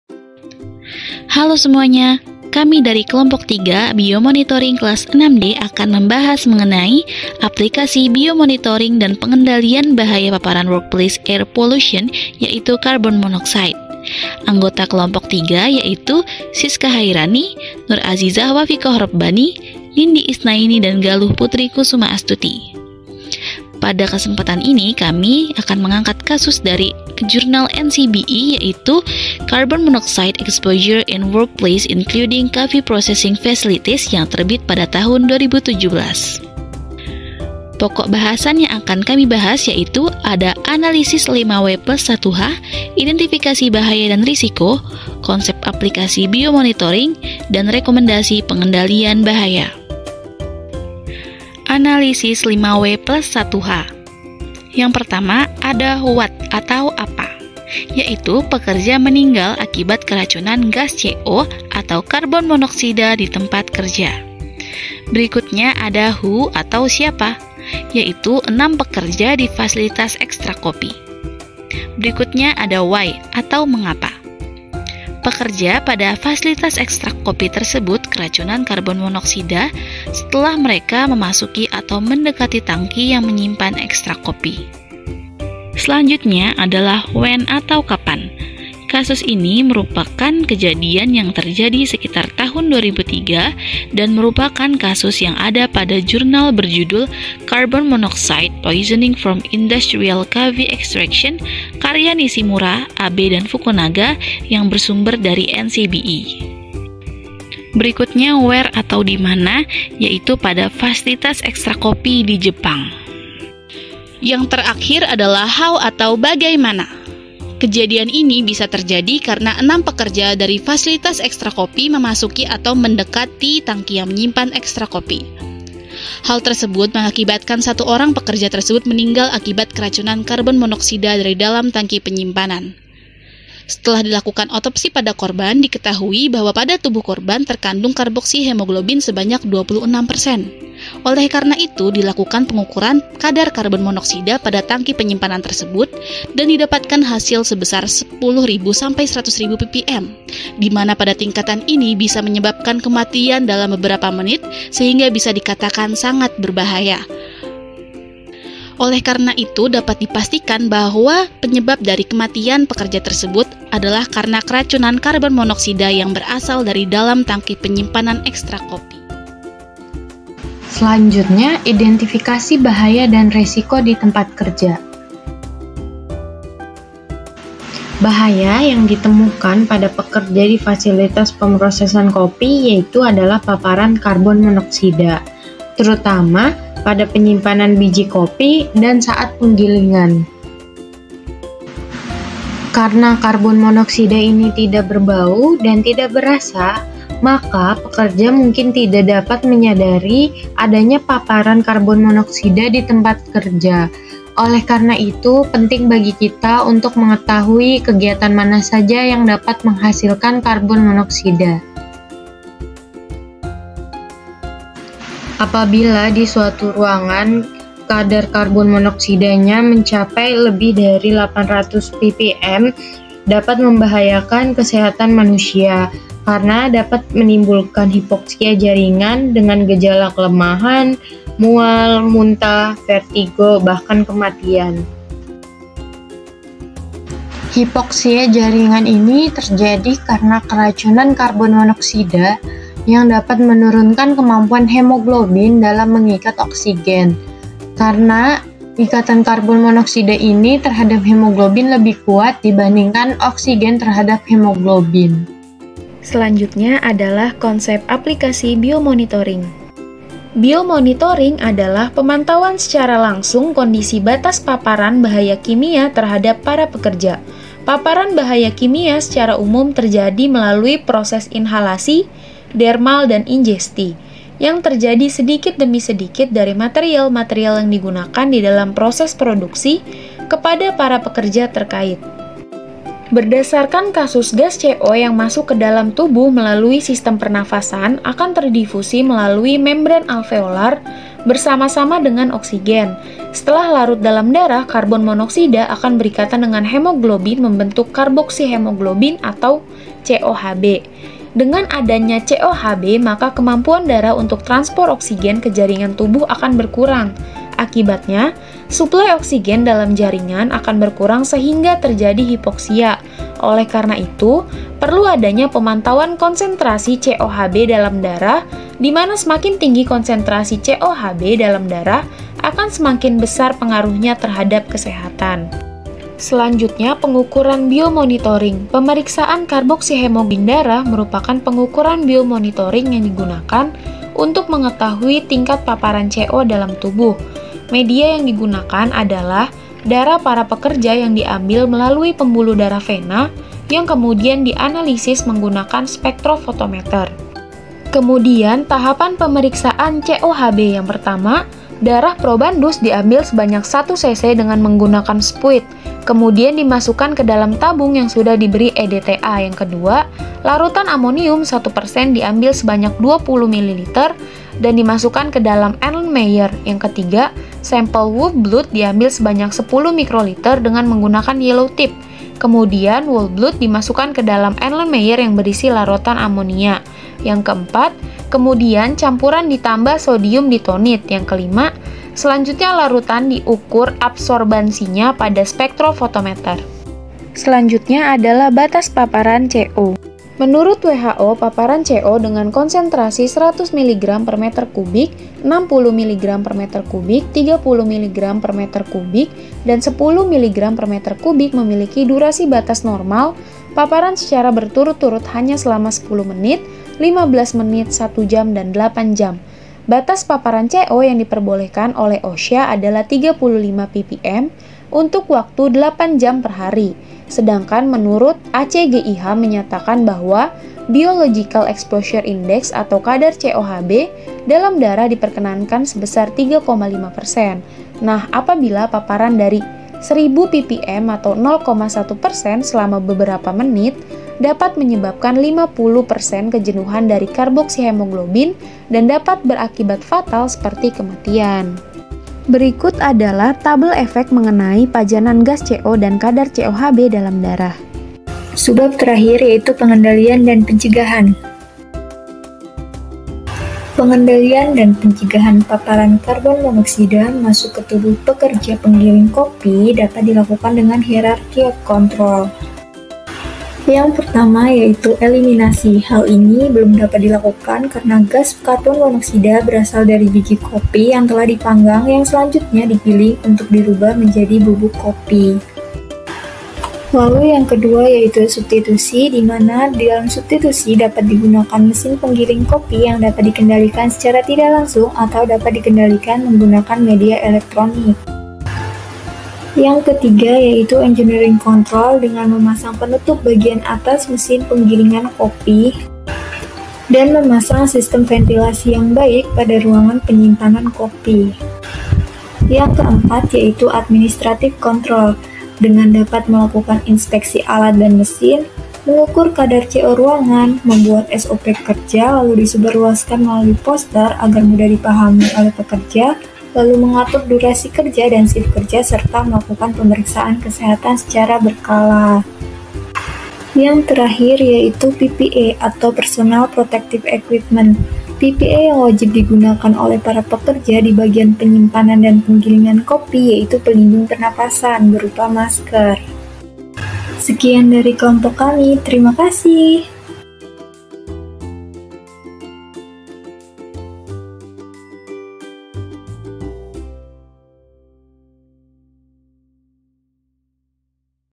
Biomonitoring dan Pengendalian Bahaya Paparan Workplace Air Pollution (Carbon Monoxide) | Case Study
Kelompok: 3
Anggota:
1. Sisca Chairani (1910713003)
2. Nur Azizah Wafiqoh Rabbani (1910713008)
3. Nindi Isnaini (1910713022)
4. Galuh Putri Kusuma Astuti (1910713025)

Kelas: 6D/ Angkatan 2019 T.A. 2021/2022
Topik: Aplikasi Biomonitoring dan Pengendalian Bahaya Paparan Workplace Air Pollution (Carbon Monoxide)
Mata Kuliah: Bahaya Kimia dan Biomonitoring

Dengan ini Kelompok 3 menyatakan bahwa video ini memang benar hasil karya dari kelompok 3, tidak ada plagiarisme dan telah mengizinkan Channel Kesmas Bela Negara untuk mengupload video berikut.